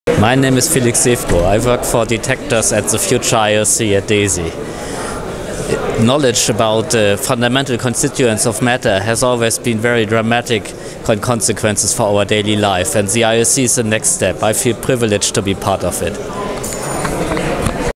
I want the ILC! by Felix Sefkow
Felix Sefkow is a physicist at DESY in Hamburg, Germany.